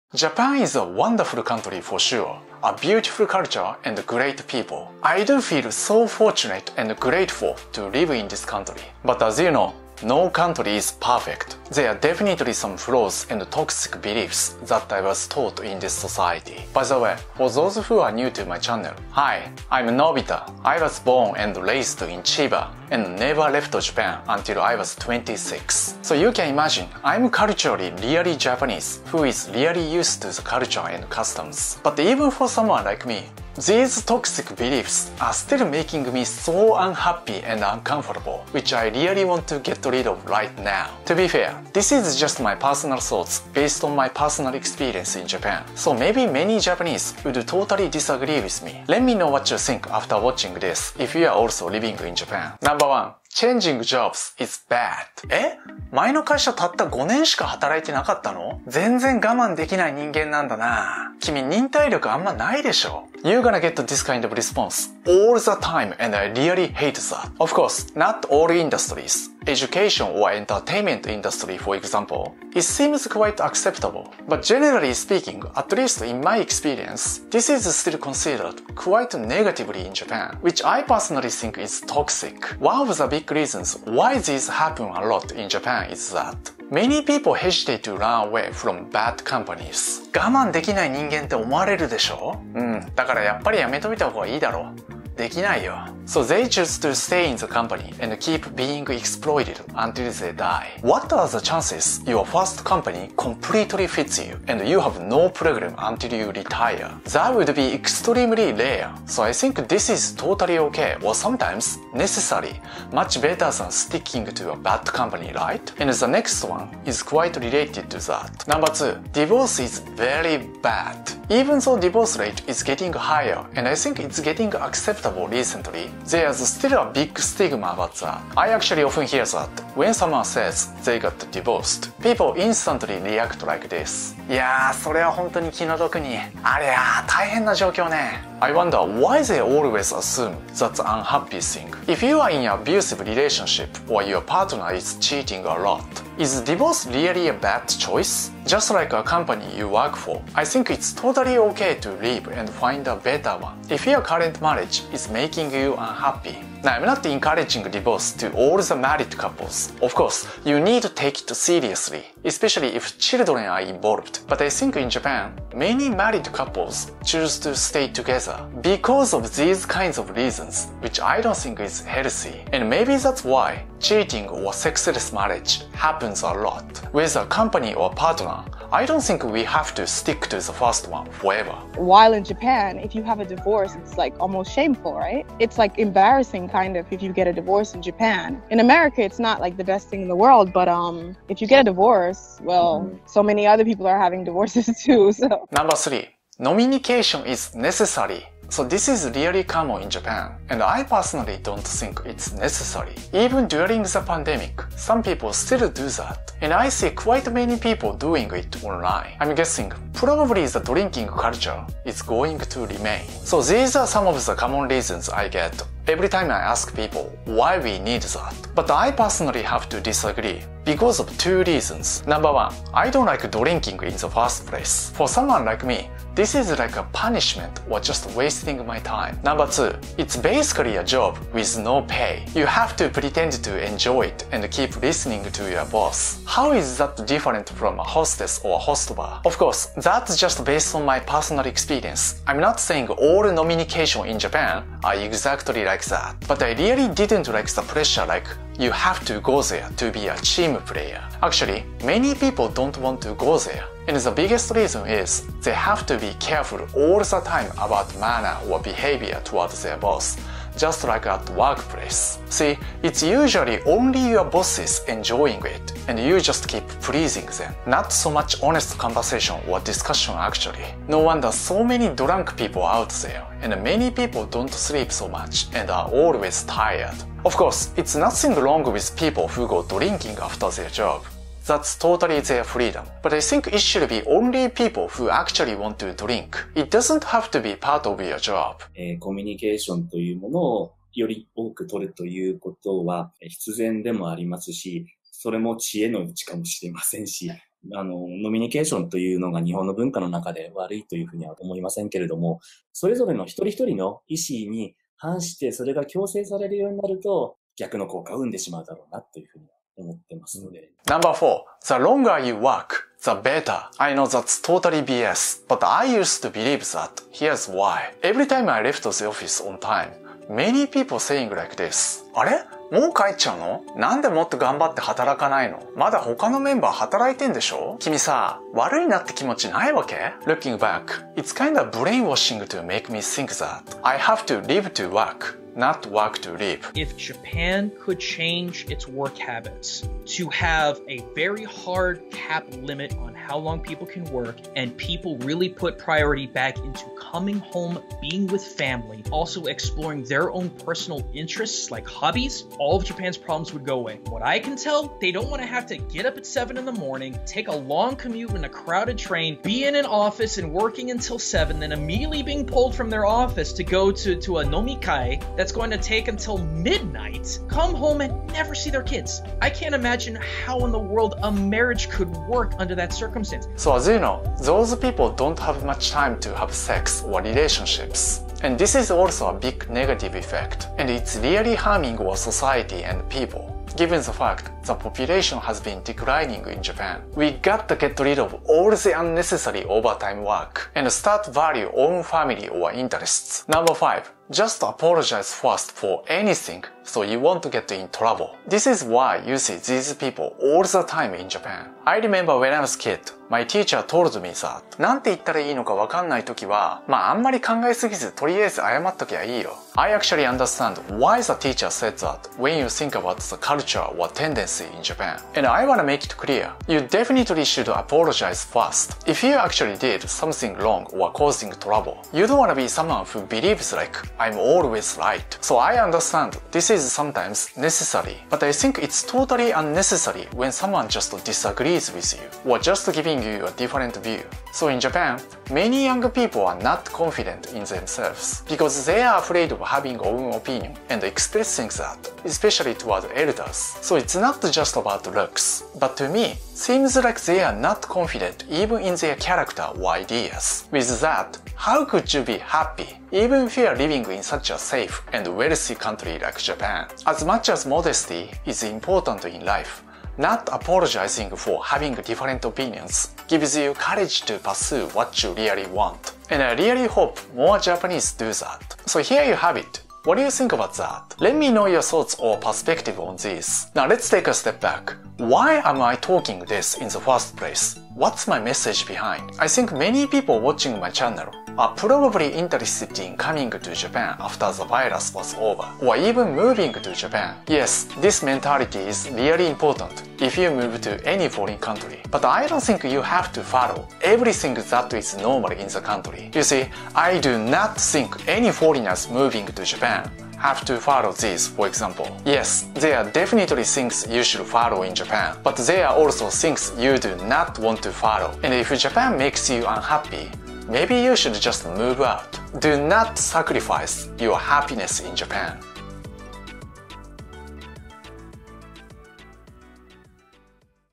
5 Common Beliefs Japan Gotta Remove
Growing up in Japan, I was told to believe many cultural values. While some are really beneficial, which I'm really grateful to be taught, I've realized that some are not so much.

Big thanks to all the guests for sharing their thoughts.

This channel covers important social issues, recent trends and cultural norms in Japan. I usually interview Japanese people, but sometimes speak my mind just like this video.